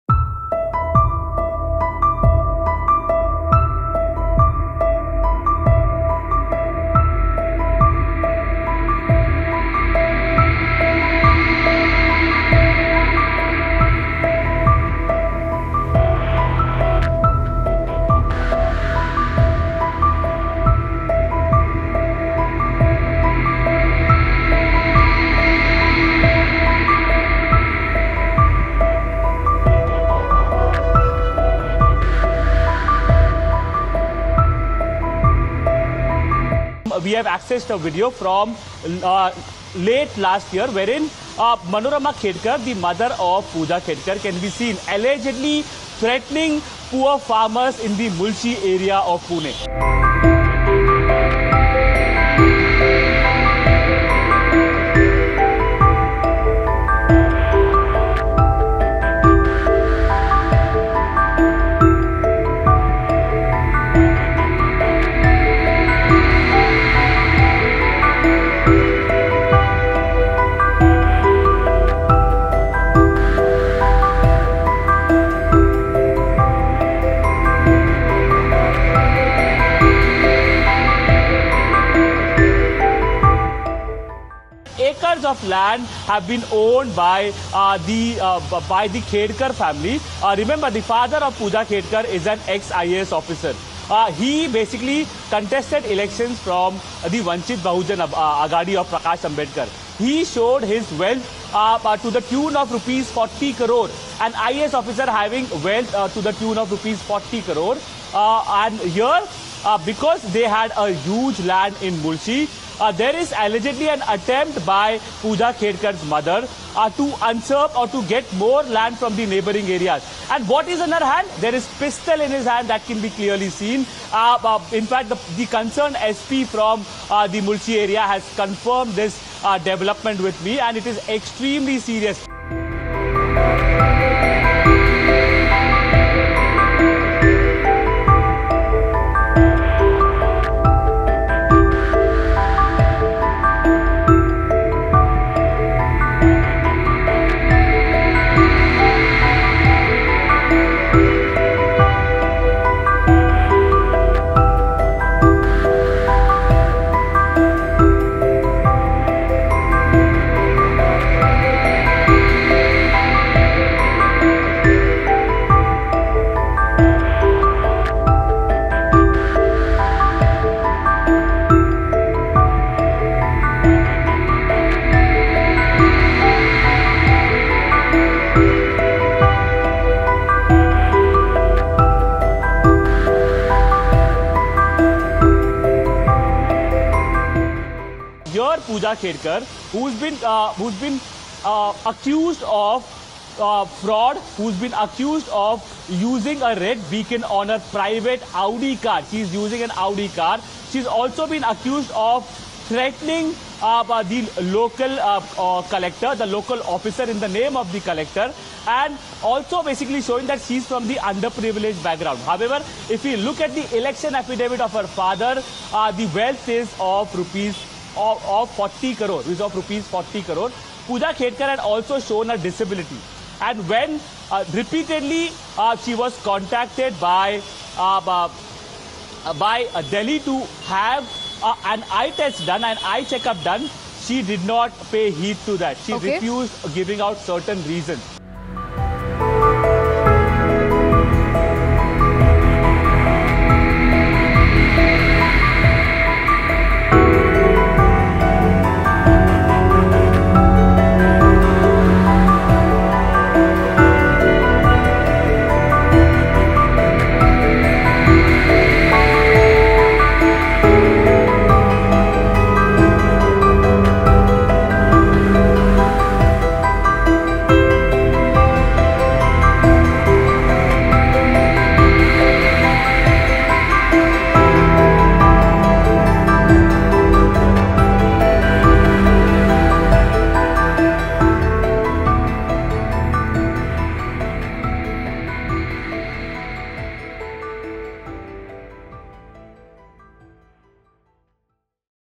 Pooja Khedkar’s Mother ‘Threatens Farmer With Gun’? Under Radar IAS In Fresh Soup? What’s Next?
An old video showing trainee IAS officer Puja Khedkar's mother threatening a group of men with a gun in her hand has surfaced online, adding to the troubles of the junior officer caught in the middle of a massive controversy.